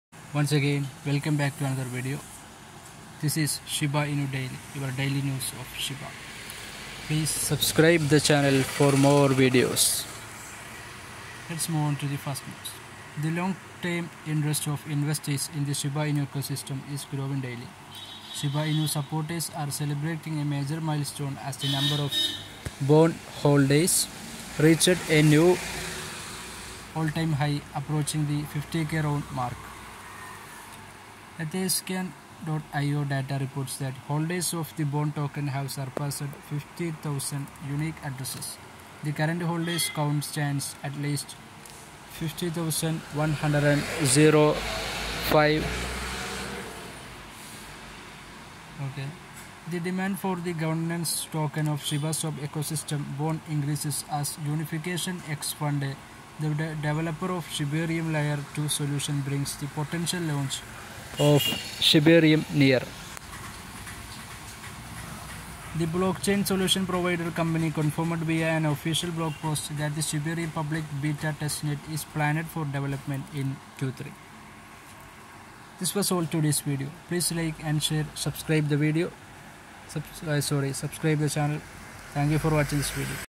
𝗦𝗵𝗶𝗯𝗮 𝗶𝗻𝘂 𝗘𝗰𝗼𝘀𝘆𝘀𝘁𝗲𝗺 𝘁𝗼𝗸𝗲𝗻 𝗕𝗢𝗡𝗘 𝗦𝘂𝗿𝗽𝗮𝘀𝘀𝗲𝘀 𝟱𝟬, 𝟬𝟬𝟬 Holders
The long-term interest of investors in the Shiba Inu Ecosystem is growing daily. Shiba Inu supporters are celebrating a major milestone as the number of BONE holders reached a new all-time high, approaching the 50K round mark.

Etherscan.io data reports that holders of the BONE token have surpassed 50,000 unique addresses. The current holders’ count stands at 50,105.

The demand for the governance token of ShibaSwap Ecosystem, BONE, increases as Unification XFUND, the developer of Shibarium – Layer 2 Solution, brings the potential launch of Shibarium near.

The blockchain solution provider company confirmed via an official blog post that the Shibarium Public Beta TestNet is planned for deployment in Q3